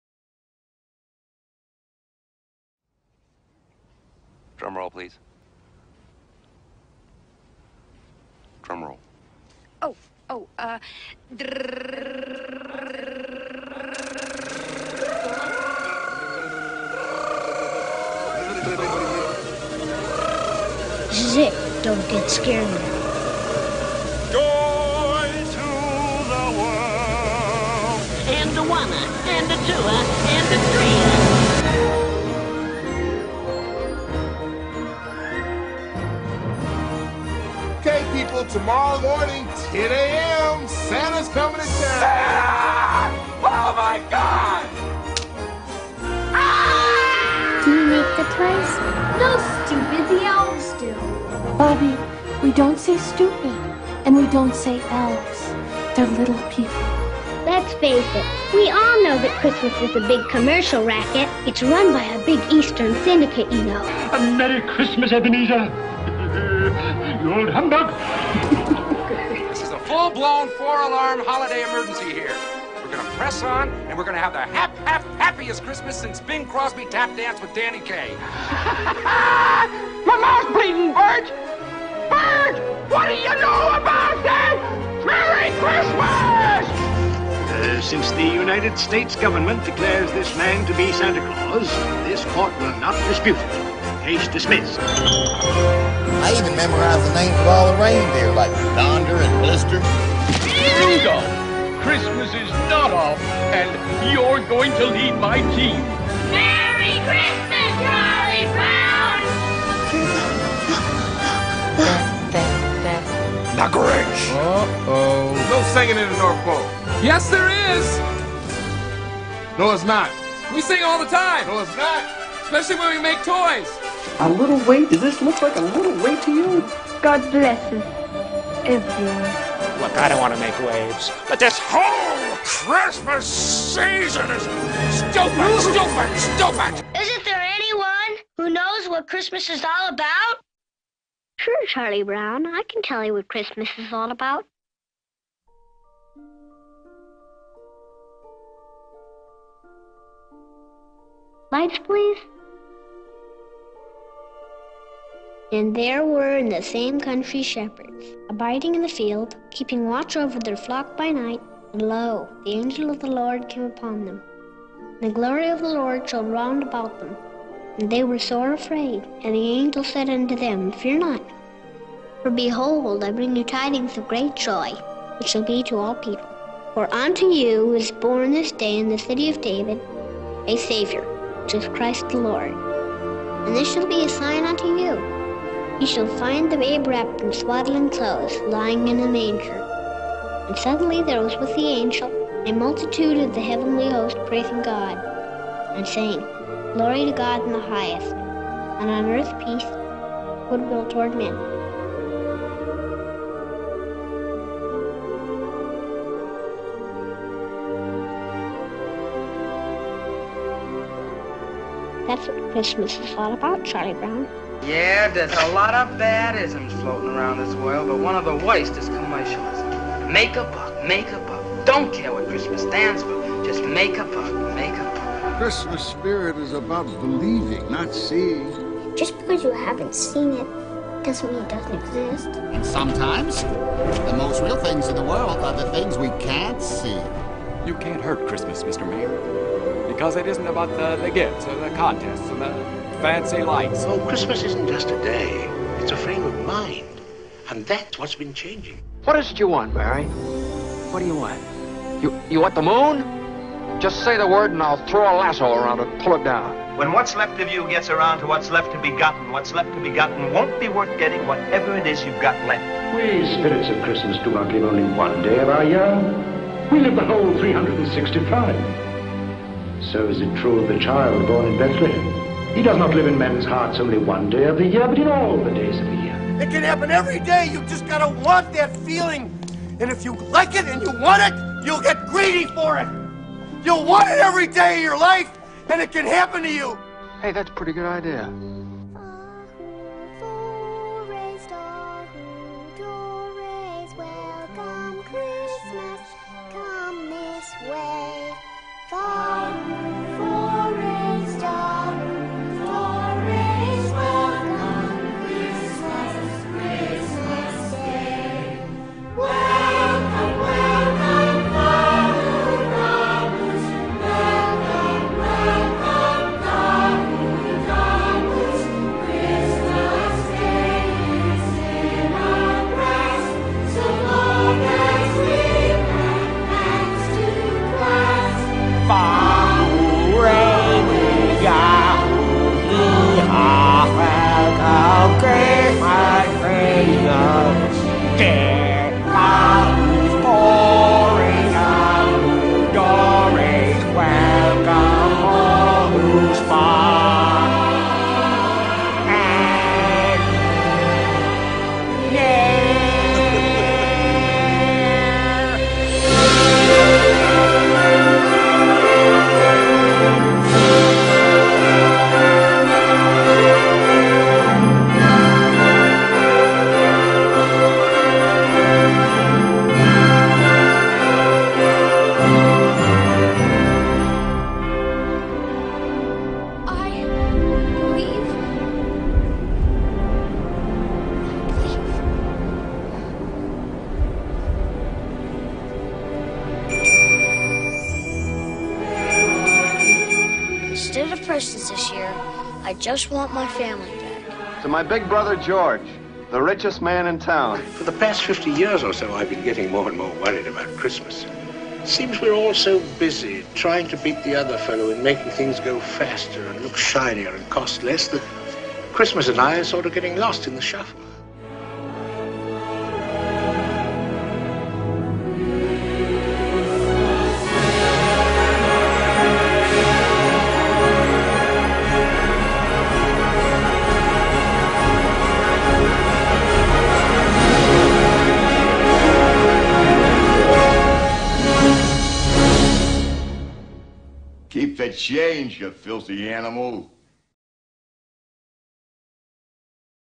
2020 Davern Family Lights - Christmas Movie Mashup
Our 2020 Davern Family Light Christmas Display on house in Tipp City/Huber Heights OH. This year we've got 50,000 all LED lights set to music and video with 160 Light-O-Rama channels! Merry Christmas!